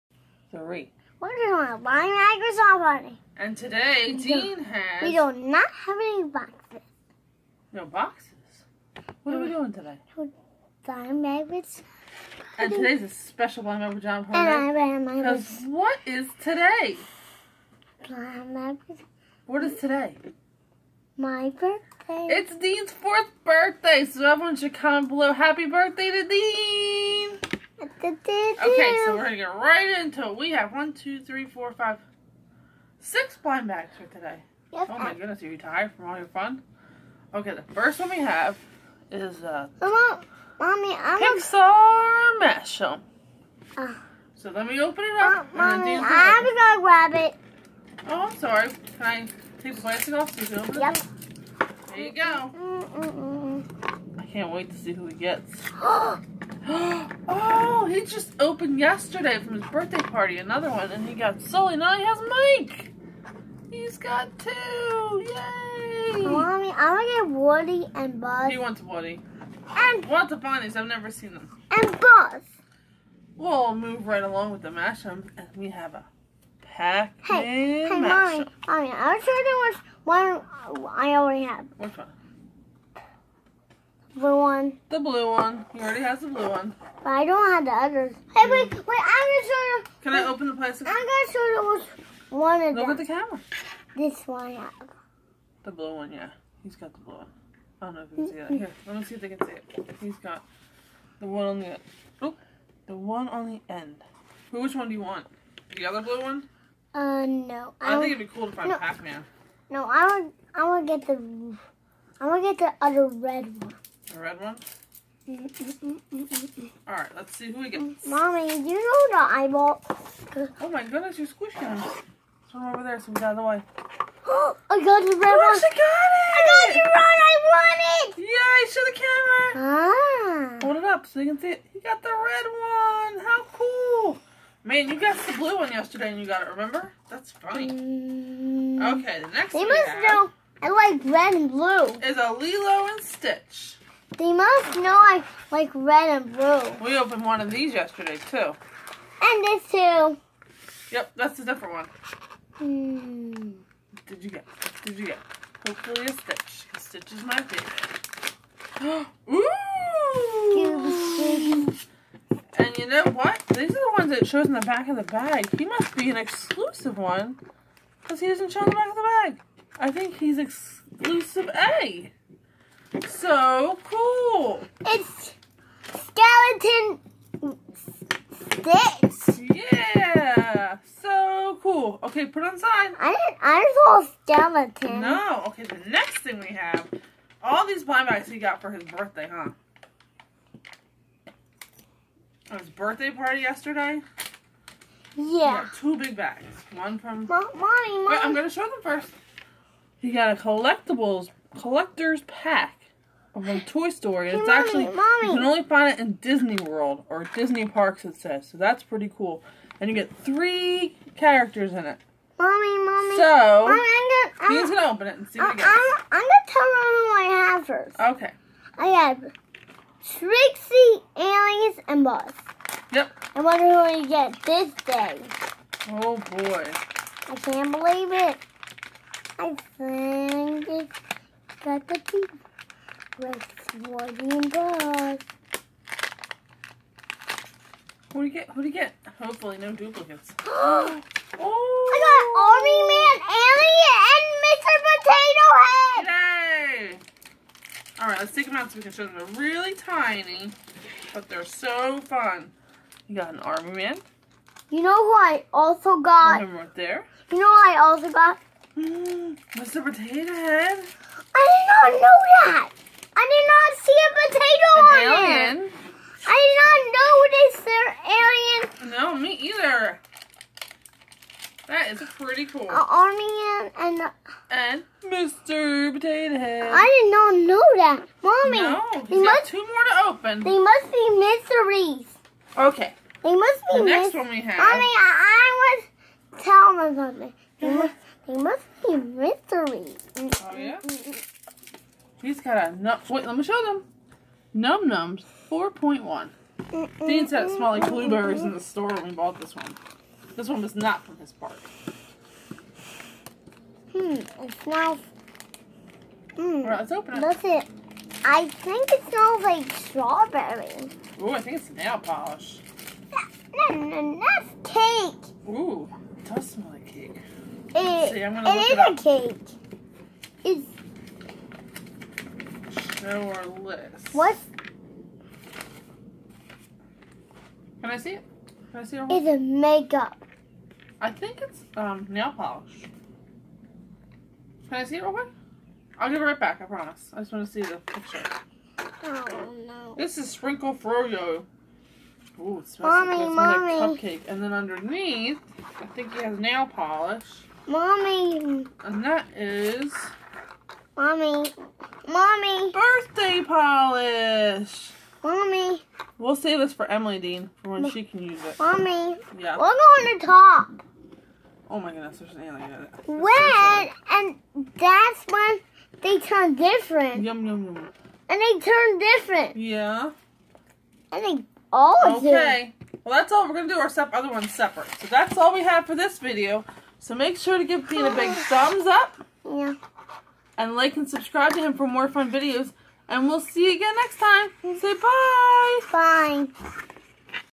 Birthday Blind Bag fun
Today is Dean's birthday! We open some fun blind bags and find an exclusive one!